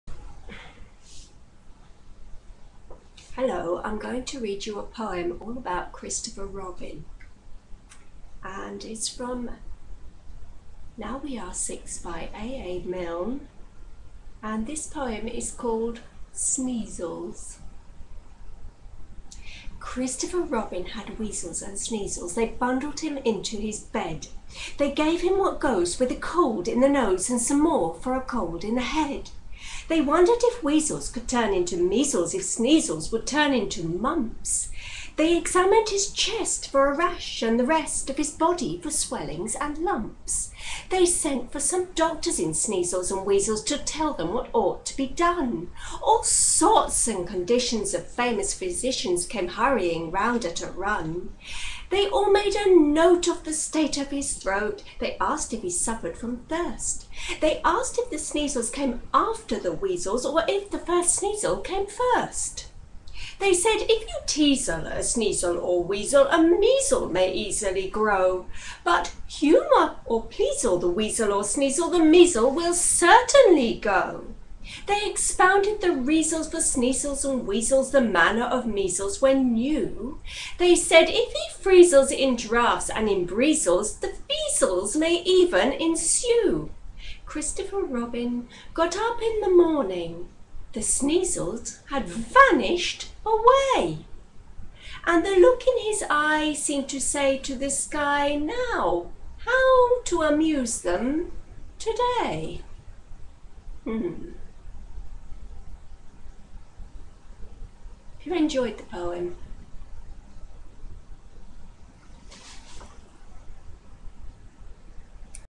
"Sneezles" from "Now we are Six" by A.A.Milne.
read by Gillian E Shaw